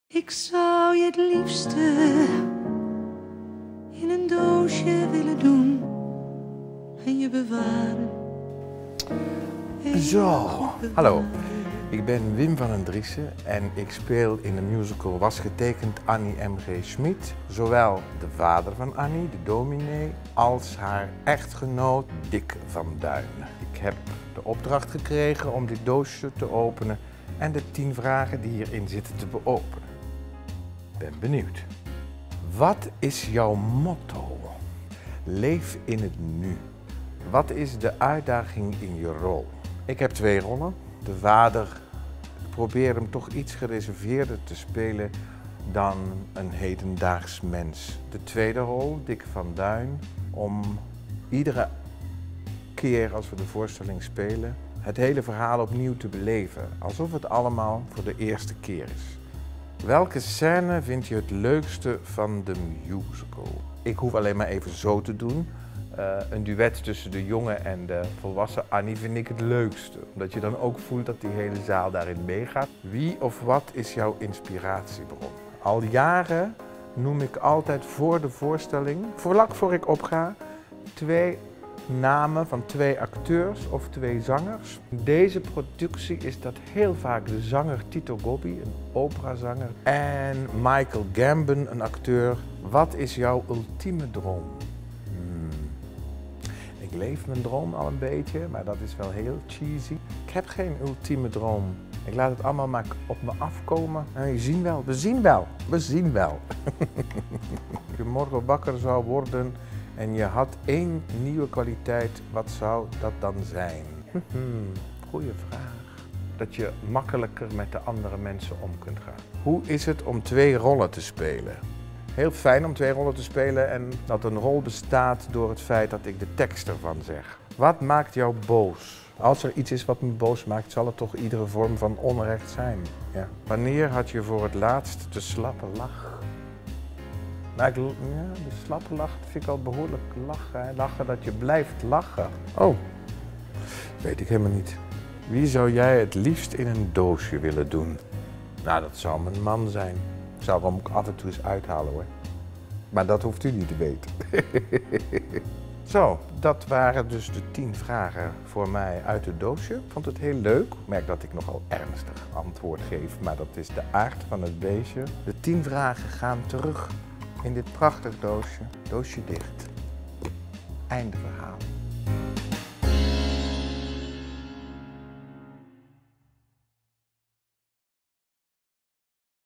Wim van den Driessche | 10 Vragen In Een Doosje
De tien vragen in een doosje zijn dit keer beantwoord door Wim van den Driessche. Hij speelt Dick van Duijn in Was Getekend, Annie M.G. Schmidt. Benieuwd naar de vragen en zijn antwoorden? Kijk dan de gloednieuwe 'Tien vragen in een doosje'.

Dit is BackStage. Voor alle ins en outs.
BackStage is het YouTube-kanaal van Stage Entertainment NL.

© BackStage - Stage Entertainment Nederland